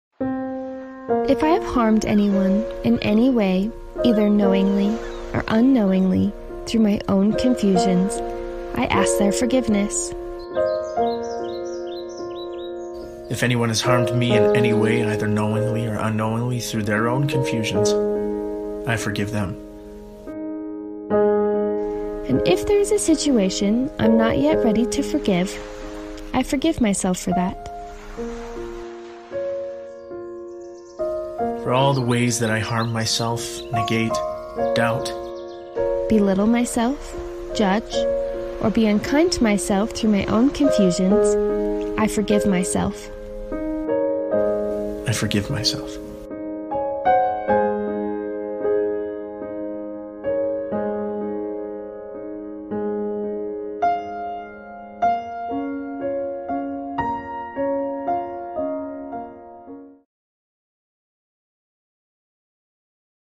The Forgiveness Prayer
Let go of grudges and open your heart to forgiveness. Join us in reciting this powerful forgiveness prayer and experience the peace that comes with letting go. 🙏

"If I have harmed anyone in any way, either knowingly or unknowingly, through my own confusions, I ask their forgiveness.

If anyone has harmed me in any way, either knowingly or unknowingly, through their own confusions, I forgive them.

And if there is a situation I am not yet ready to forgive, I forgive myself for that.

For all the ways that I harm myself, negate, doubt, belittle myself, judge, or be unkind to myself through my own confusions, I forgive myself."